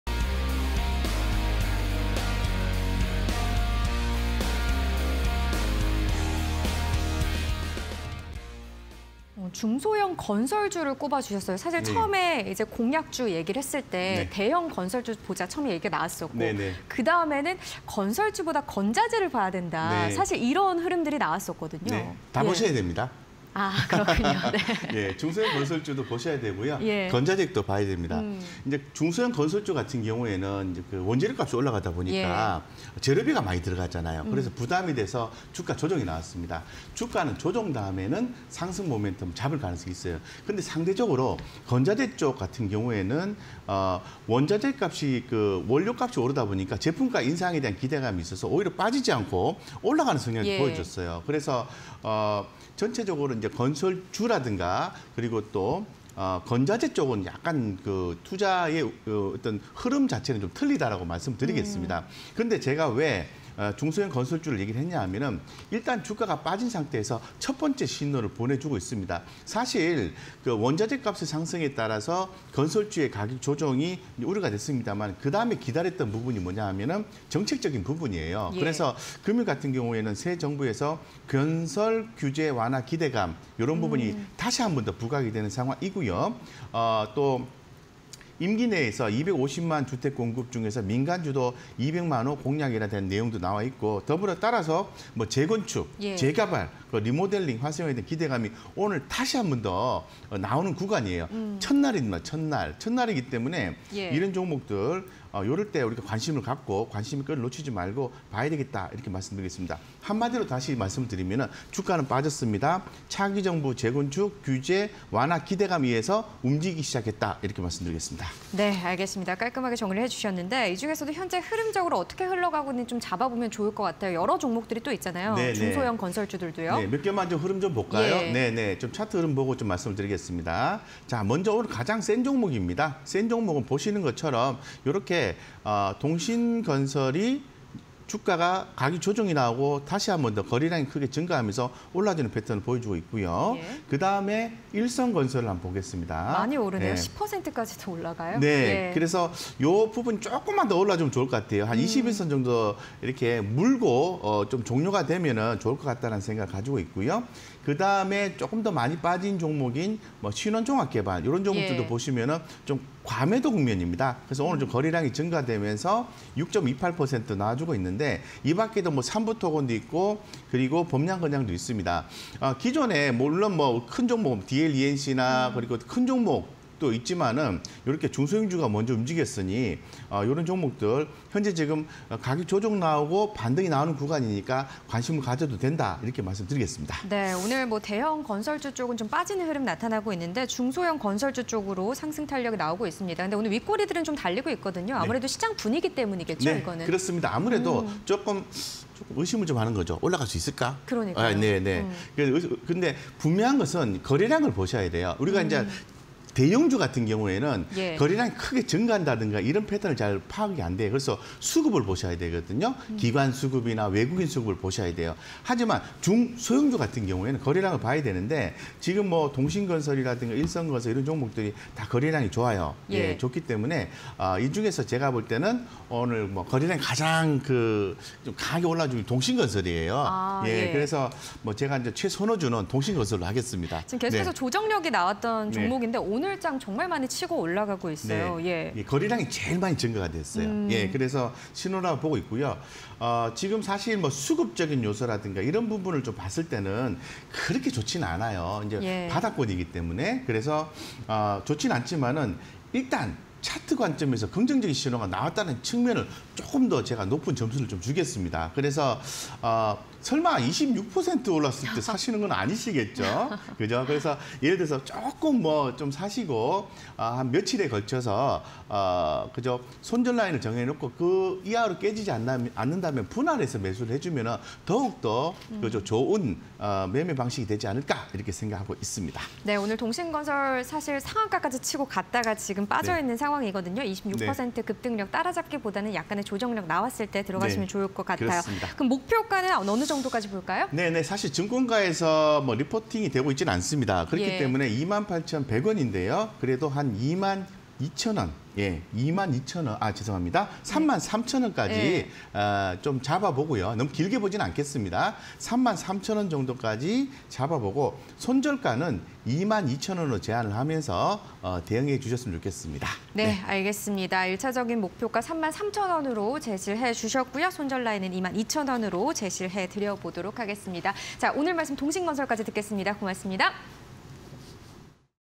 동신건설(025950)... 중ㆍ소형 건설주_테마IN이슈 (20220415)
# 중ㆍ소형 건설주
- 새 정부, 건설 규제 완화 기대감에 상승
- 尹 "250만호 주택 공급 중 민간주도 200만호" 공약
- 재건축ㆍ재개발ㆍ리모델링 활성화 기대감

동신건설(025950)
- 국내 도급공사 위주 건설업체
- 실내 인테리어 건자재 관련 실적 개선 기대
- 주가 상승 추세 유지에 '무게'
- 금일 거래량 동반한 상승 긍정적